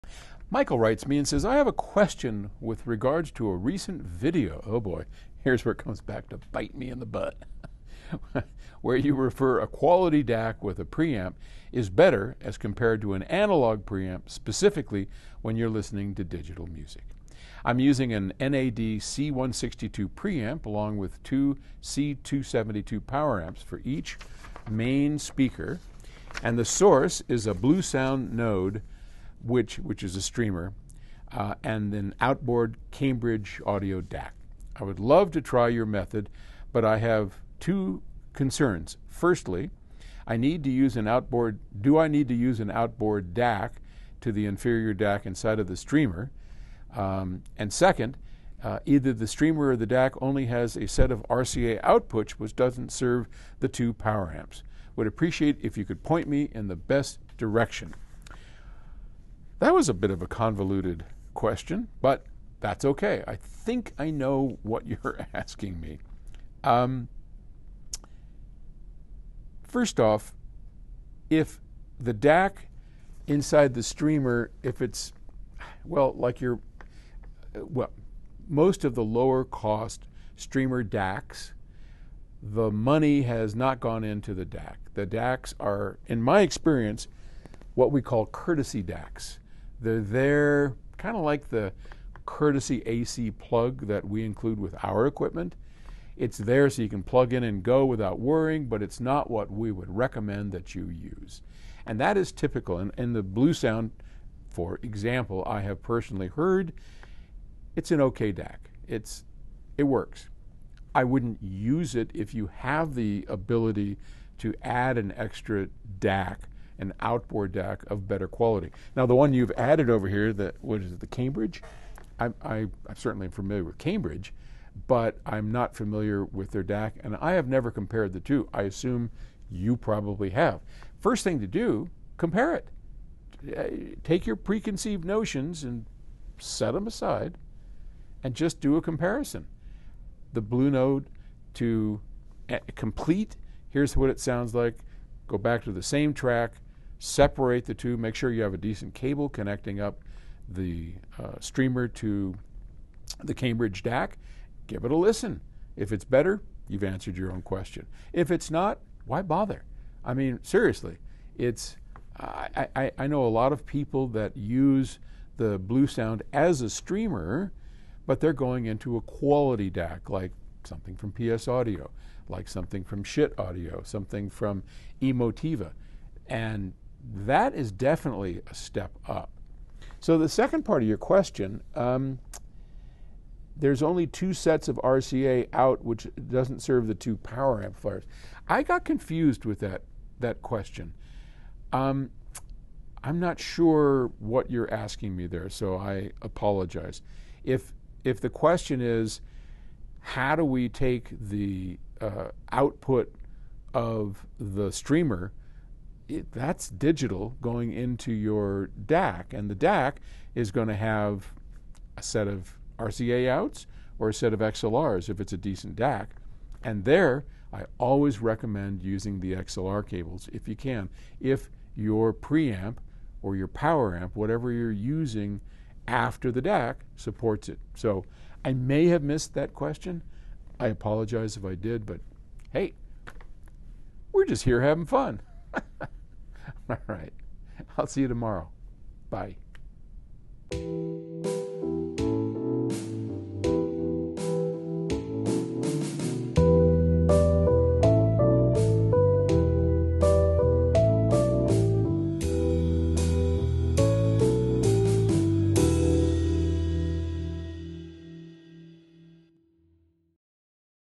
DAC with and without a preamp
Why does one even need a preamplifier in an all digital system? Paul helps us understand.